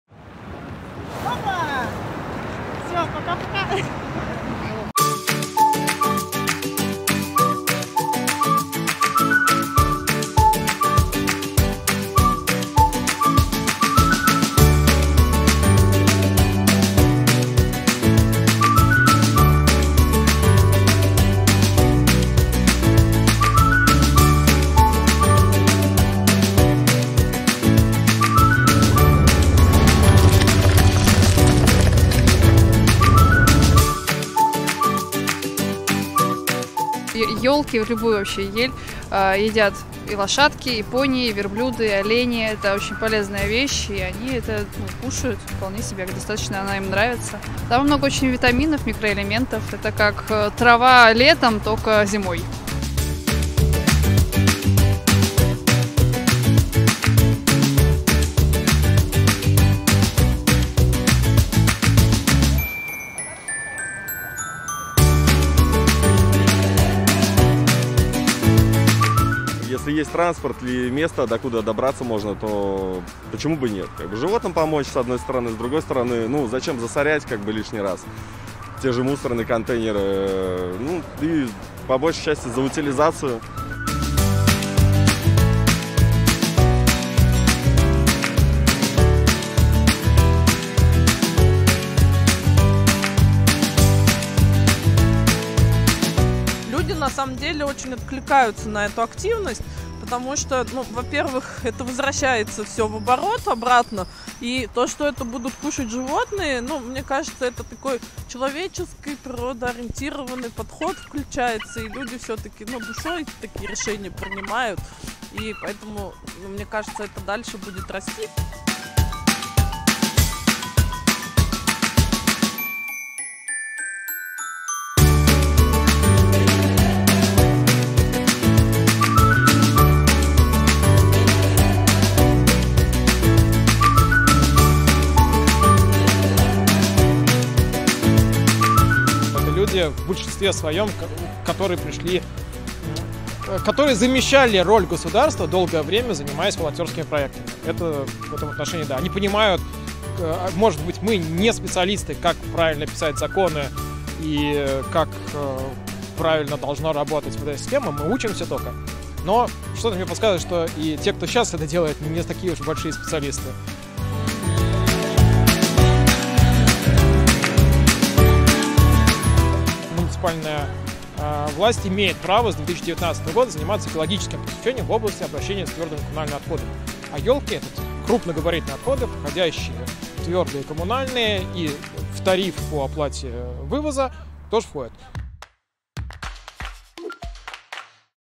В Петербурге придумали как утилизировать новогодние елки
В Санкт-Петербурге волонтеры превращают старые елки в корм и подстилку для животных. В январе деревья собирают на специально оборудованных площадках, затем сортируют и измельчают. Получившуюся щепу и хвою отдают в приюты и конюшни.